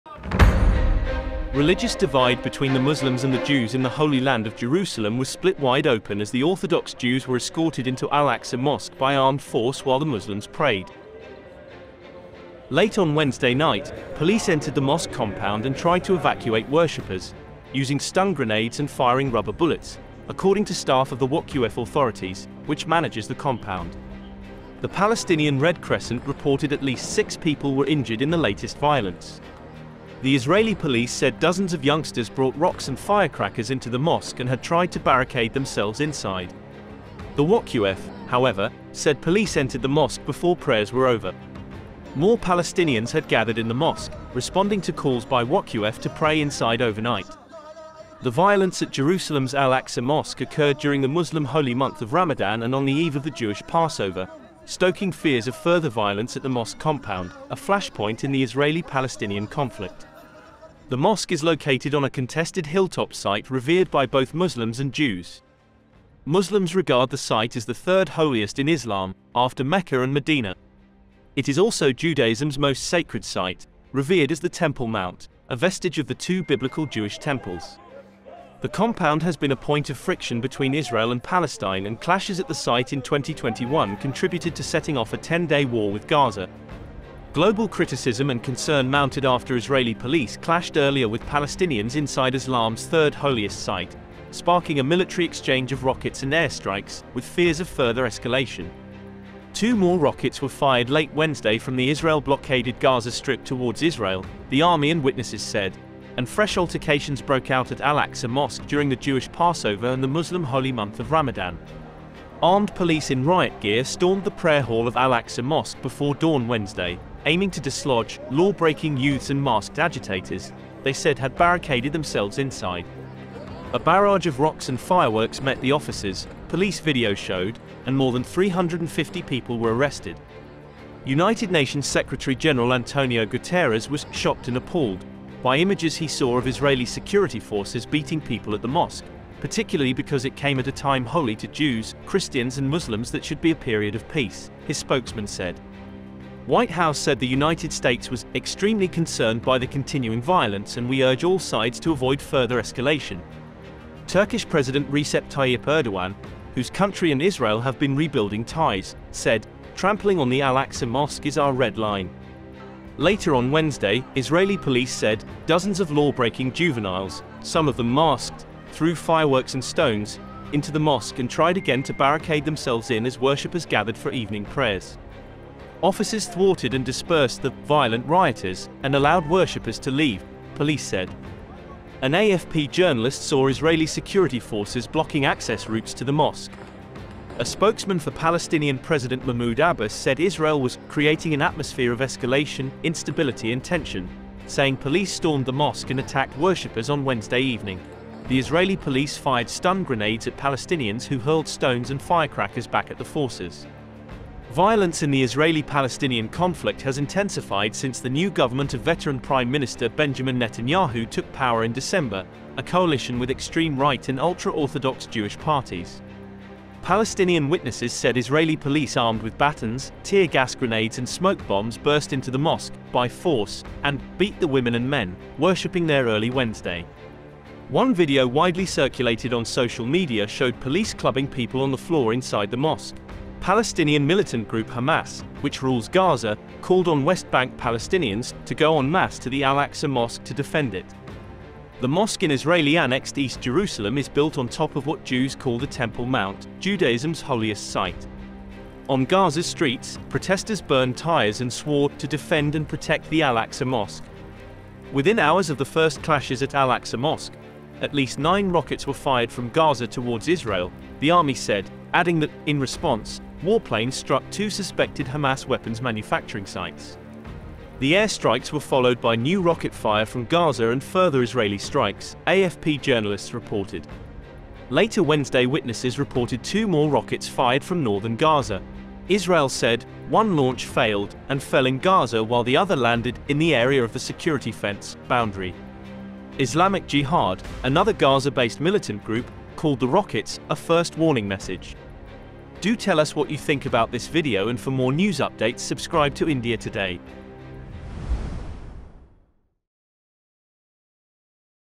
Israeli Police & Muslim Worshippers Clash Again At Al-Aqsa mosque During Jewish Passover & Ramadan
Description: Israeli forces evacuate Palestinians from the Al-Aqsa mosque compound through Jerusalem's Old City's Chain Gate, after overnight clashes broke out when armed police in riot gear stormed the prayer hall of Al-Aqsa mosque before dawn Wednesday. Global criticism and concern mounted Wednesday after Israeli police clashed with Palestinians inside Islam's third-holiest site, sparking a military exchange of rockets and air strikes, with fears of further escalation.
Two more rockets were fired late Wednesday from the Israel-blockaded Gaza Strip towards Israel, the army and witnesses said, and fresh altercations broke out at Al-Aqsa mosque during the Jewish Passover and the Muslim holy month of Ramadan.